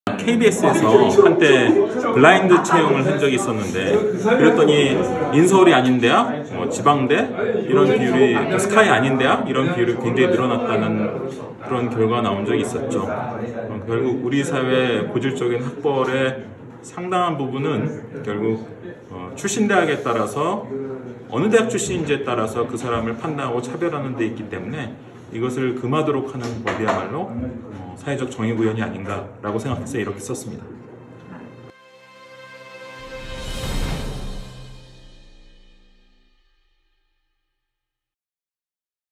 출.차.법 응원 영상 5탄 - 이범 교육평론가
출신학교 차별금지법 제정을 위한 이범 평론가의 응원 영상입니다.